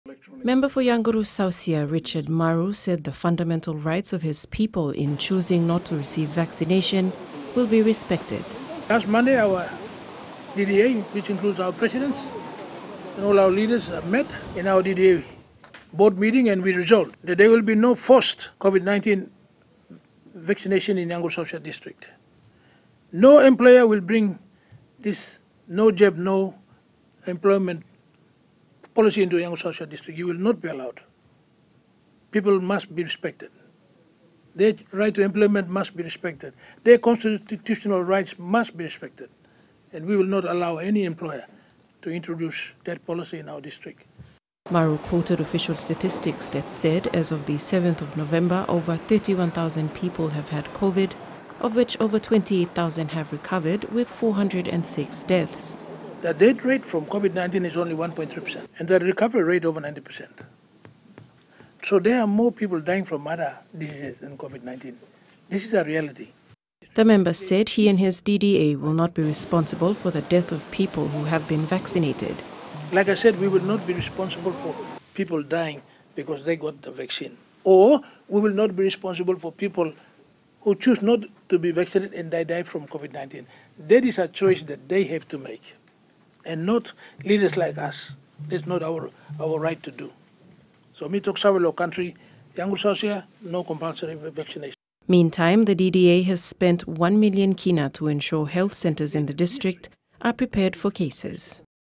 Vaccination Remains Voluntary
The Member for Yangoru Saussia and leader of the People First party, Richard Maru says that COVID-19 vaccination and the “No Jab, No Job” policy will not be forced upon the people of Yangoru Suassia District.

 This was decided at a meeting of the Yangoru District Development Authority recently.